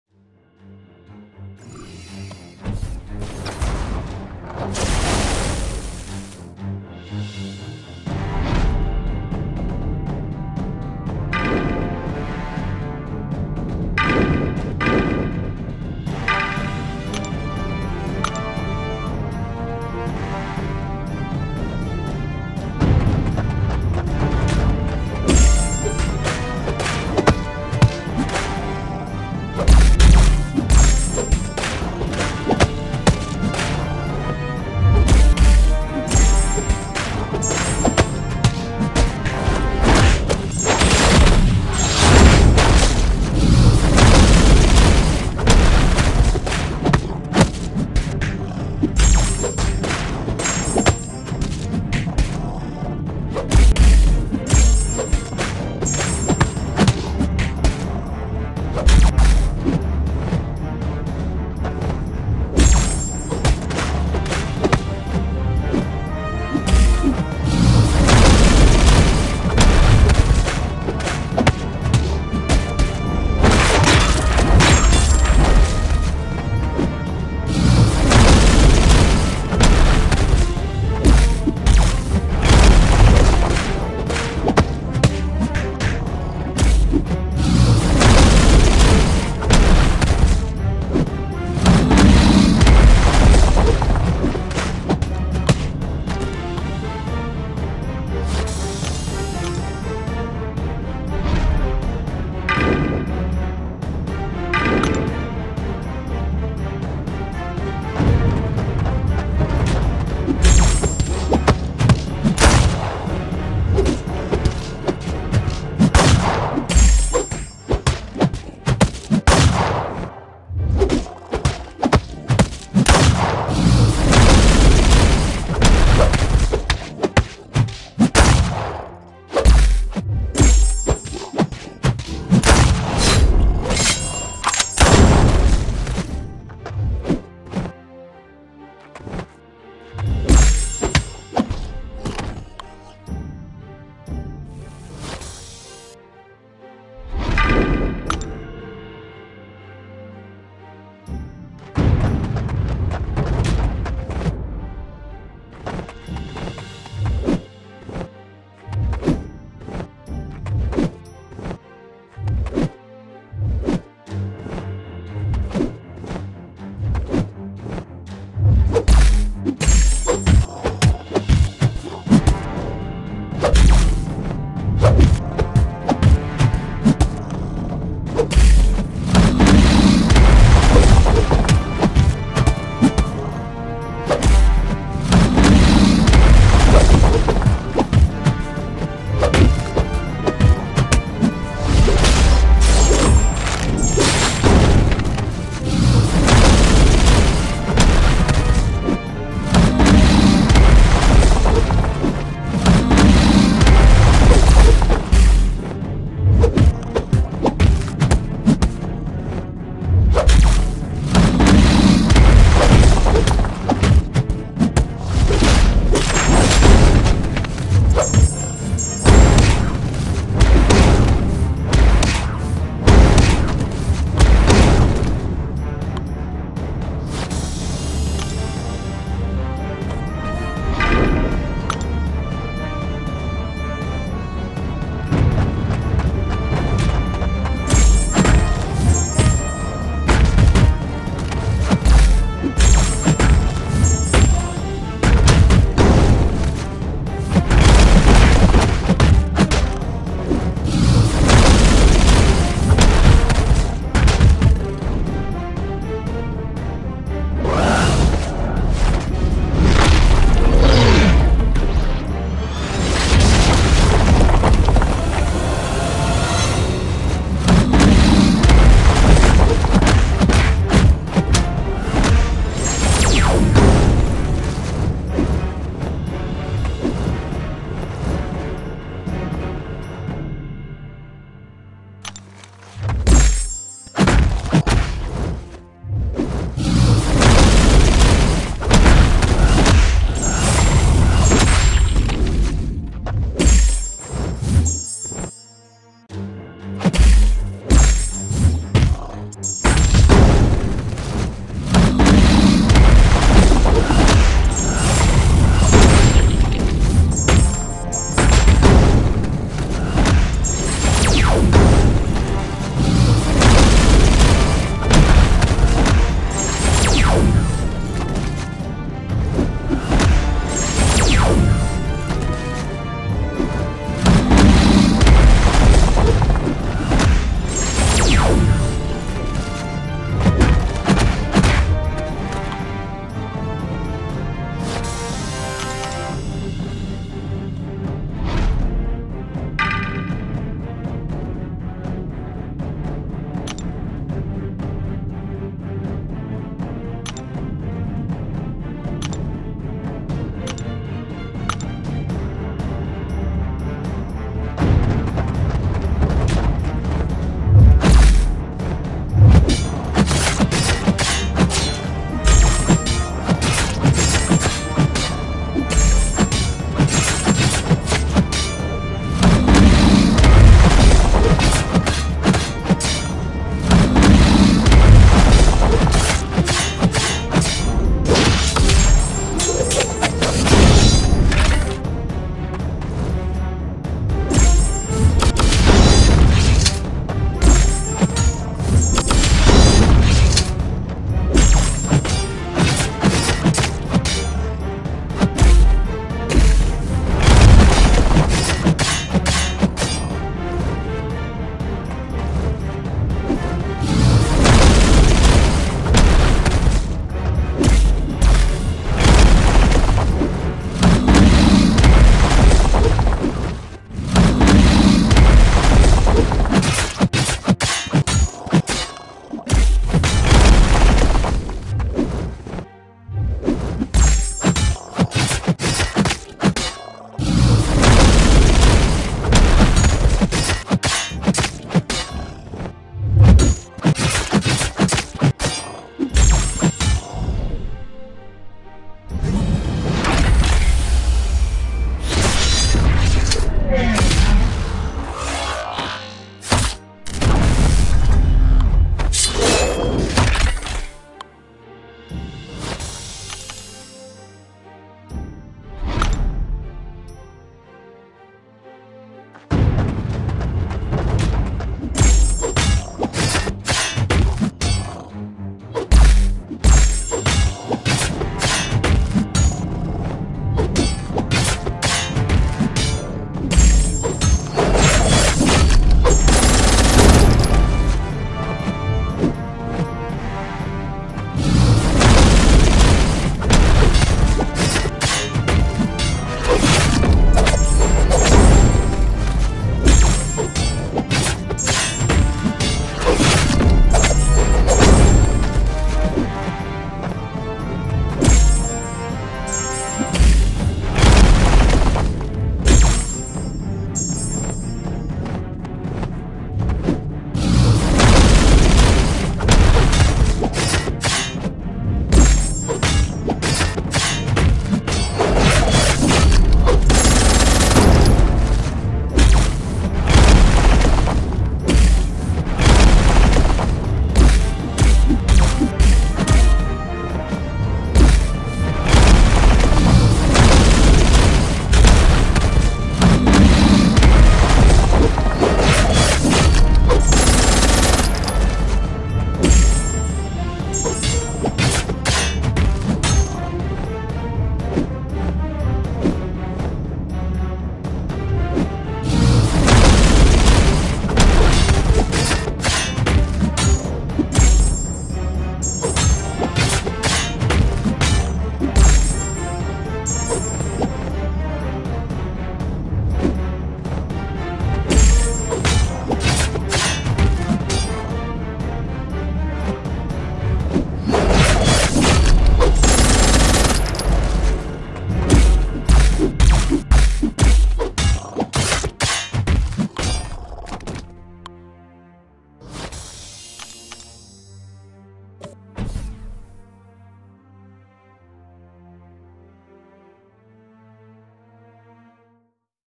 Canadian Sasquatch Hunting - We've Got a Wild One
Just a couple of fights against Sasquatch in Northern Expeditions Canadian difficulty using Nick Fury, Stealth Suit Spider-Man, Hyperion, Guillotine 2099, Stark Enhanced Spider-Man, and Blade.